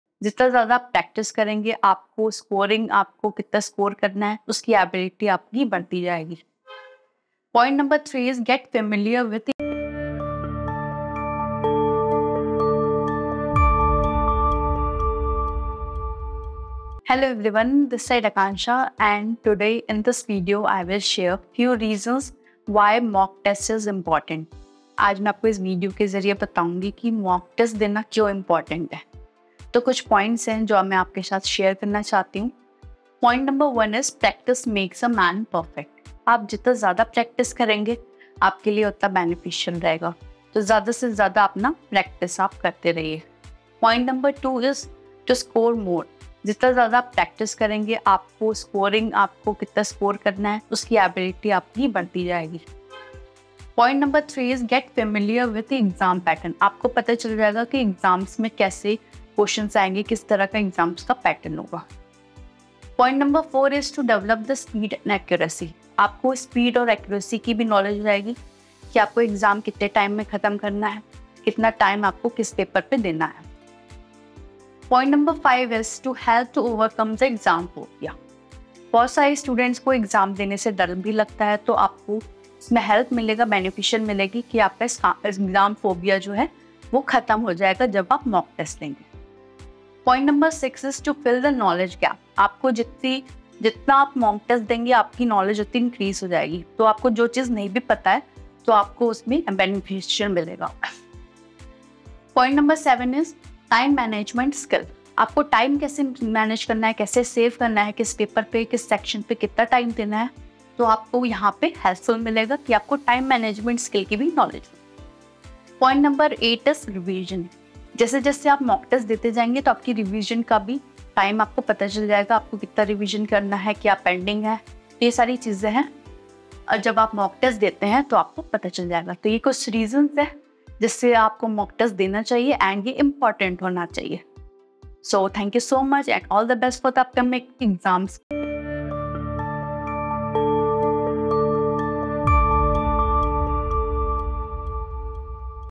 Why Mock Tests are Important | Reasons For Taking Mock Test | UPSC Preparation 2024 | PSIR CLUB
In this video, we explore the vital significance of mock tests and why they should be an integral part of your exam preparation strategy. Mock tests serve as invaluable tools for gauging your readiness, allowing you to identify strengths and weaknesses in your knowledge and test-taking skills. They simulate the actual exam environment, helping you manage time efficiently, build confidence, and reduce anxiety. Moreover, mock tests provide an opportunity to practice applying learned concepts, increasing retention. By watching this video, you'll gain a deeper understanding of why mock tests are essential, ultimately enhancing your chances of success in any exam you undertake. Don't miss this insightful guide to exam readiness!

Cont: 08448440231